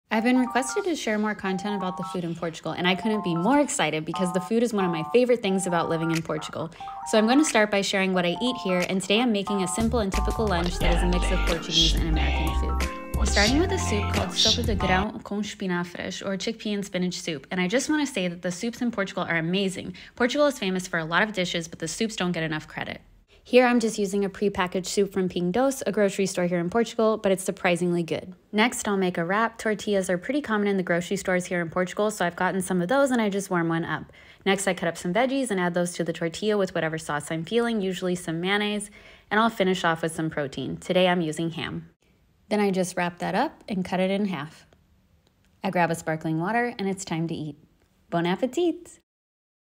A Typical Lunch For Me In Portugal
Make lunch with me at home in Portugal in my first video about what I eat here in Portugal!

While there are many amazing recipes and restaurants, most of what I eat here is simple, easy-to-prepare meals with a Portuguese flare. Like and follow for more content on food and everything else in Portugal 🇵🇹

💬 Want to know if it’s possible to find a certain good in Portugal?

LINKS*
Cutting board